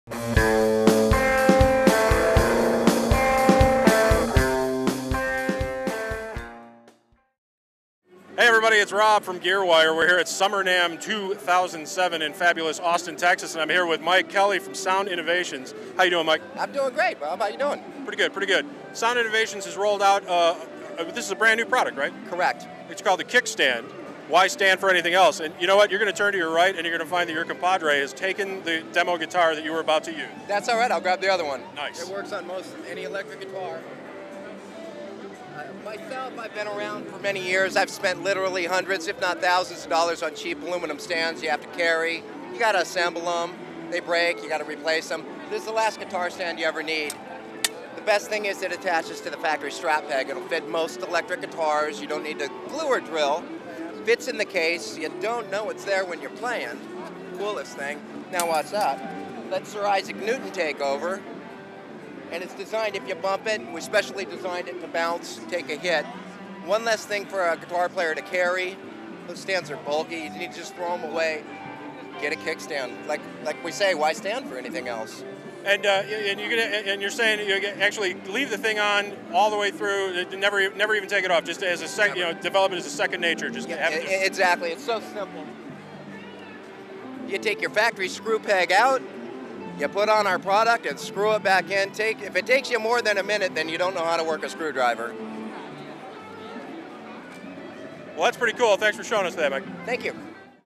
Kickstand: New Product Attaches Directly To Guitar Or Bass
Ever needed to pack all your guitar equipment into one  case for travel?  Need a new way to make your airport security experience less of a hassle?  Kickstand seems to be the appropriate solution for both cases.

Simply by attaching the Kickstand to your existing hardware allows you to have an instant guitar stand.  It stays attached while playing; barely noticeable and definitely unobtrusive.  When you are done playing, simply swing it out and set down your guitar.  This stand is especially designed to bend back if the guitar receives any impact.